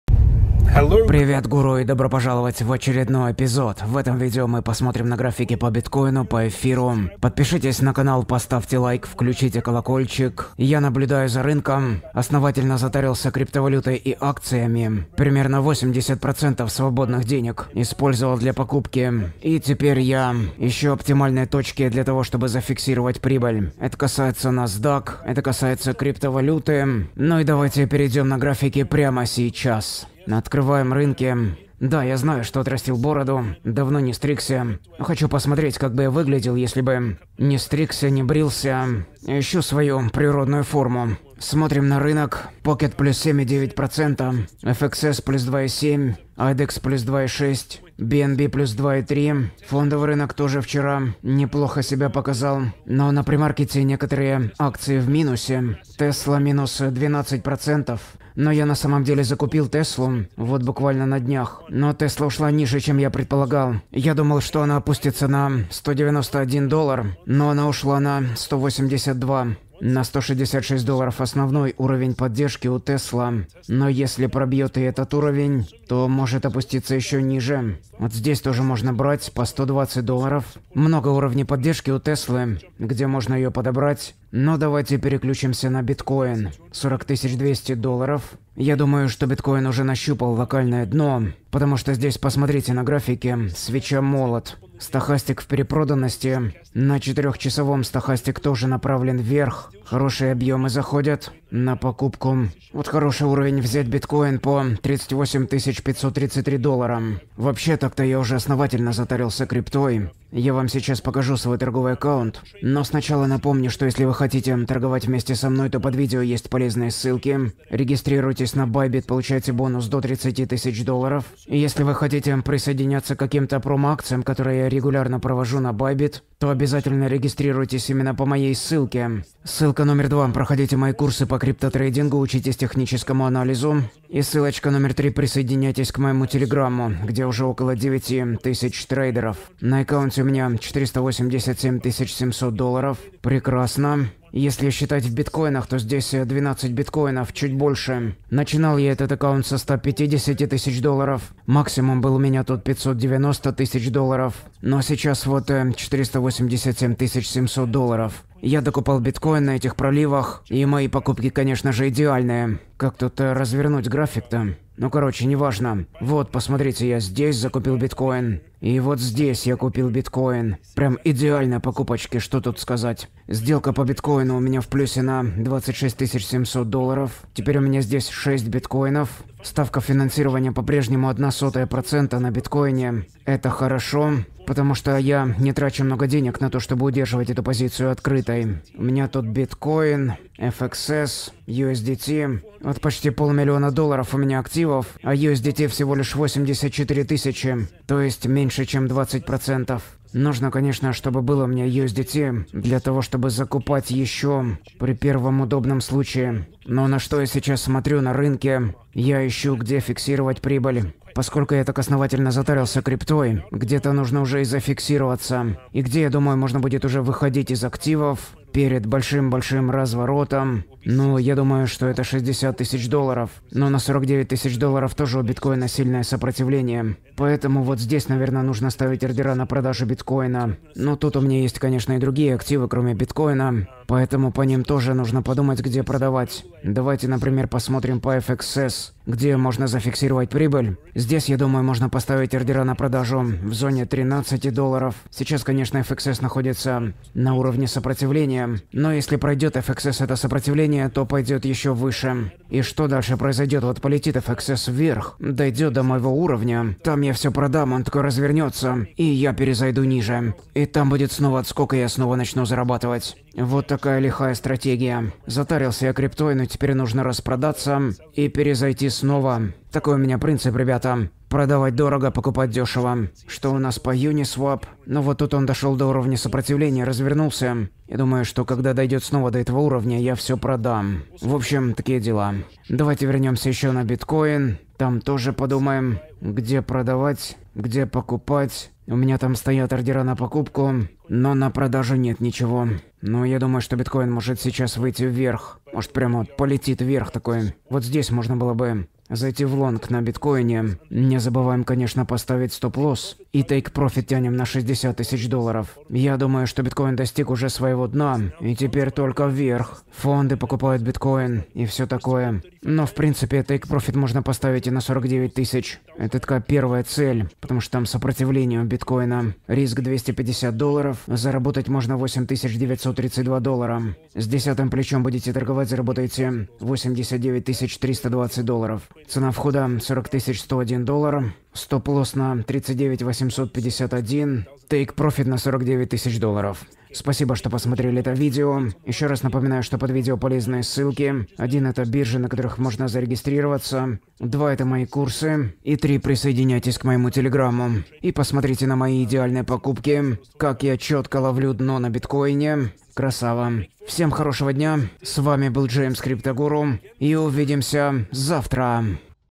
Не верь манипуляциям!⚠️Будет рост!⚠️Технический анализ биткоина
Думаю, что они просто хотят заставить вас продавать биткоин, и я расскажу почему!

Дисклеймер: Это не финансовый совет. Проводите свои исследования.